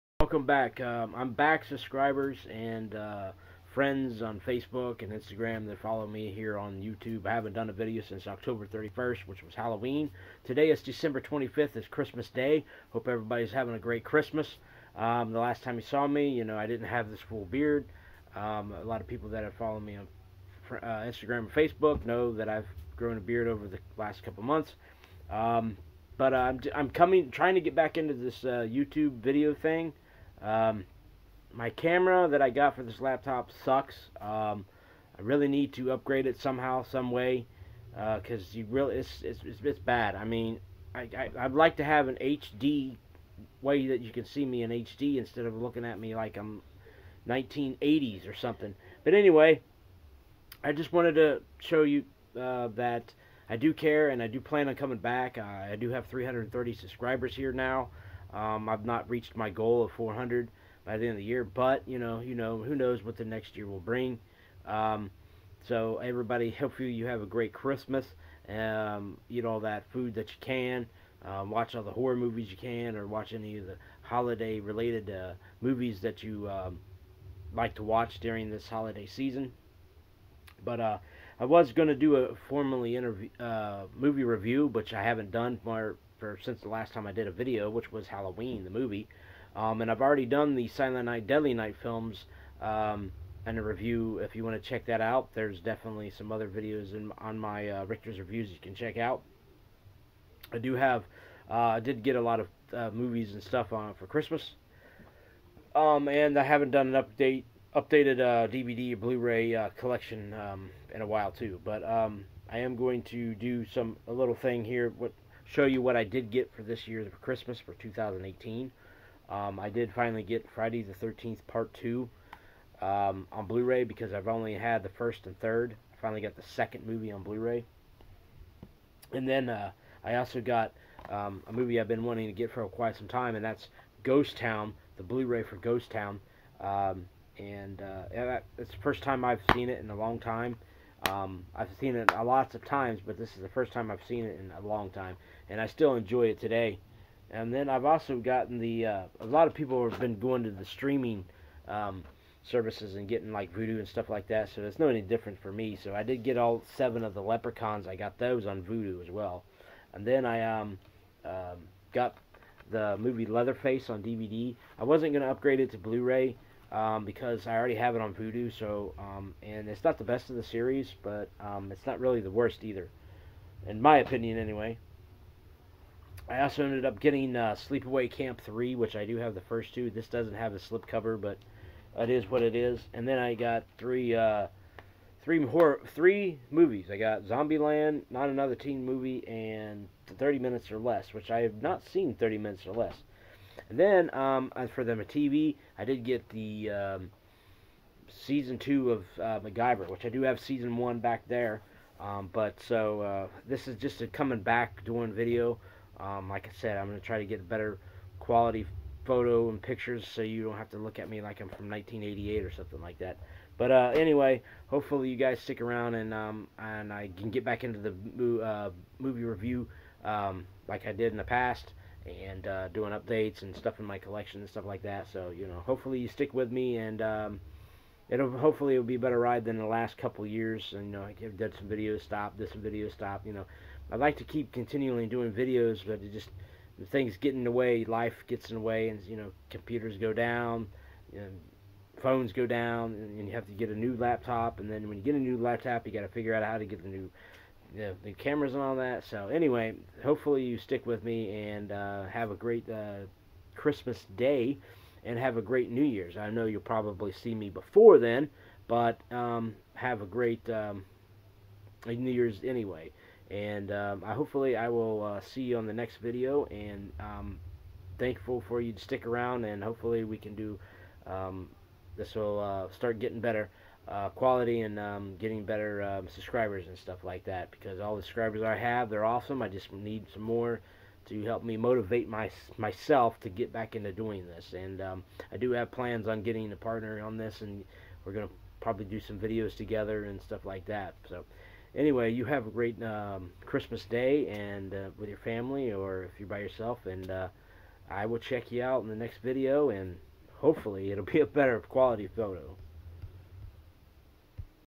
Merry Christmas!!!!
What I Got For Christmas! Glad To Be Back!! Tomorrow I will Be Back Doing Movie Reviews!!! Thank You To Those That Stayed!!